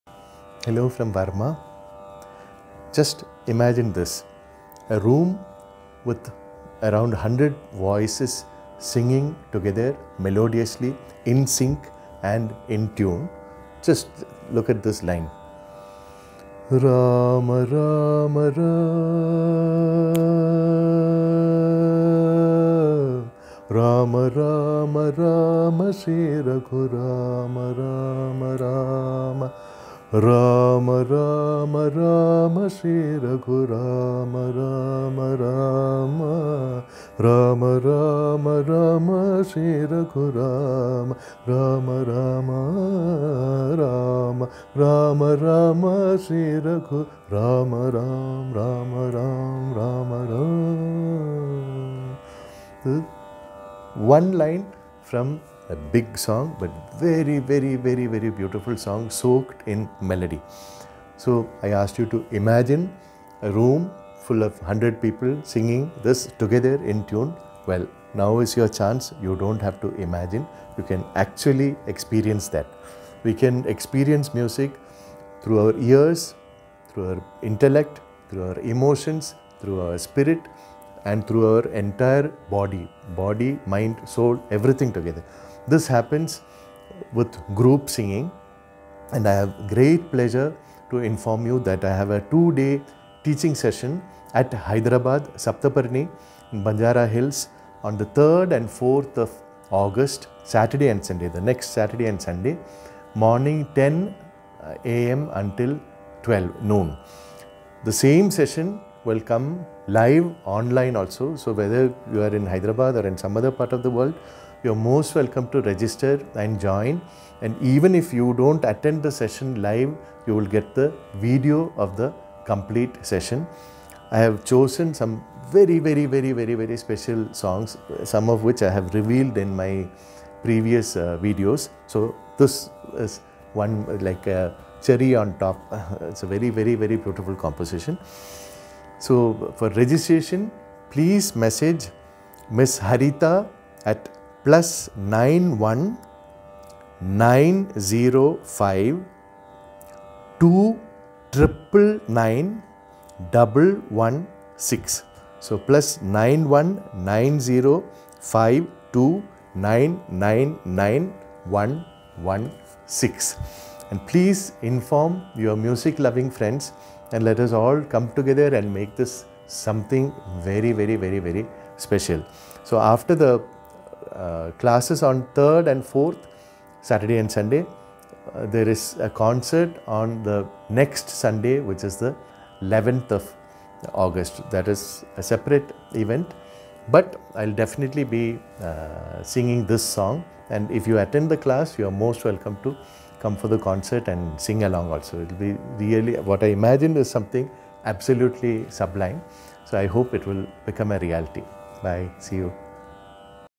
Prince Rama Varma - Special Singalong Session!
It is proposed to have a two day teaching session by Varma ji on the 3rd and 4th of August (Saturday and Sunday) from 10:00 am to 12:00 noon, at Saptaparni, Banjara Hills, Hyderabad.
This is being organized by Ms.Haritha. These sessions come online too and the video of the complete session would be made available to everyone who registers.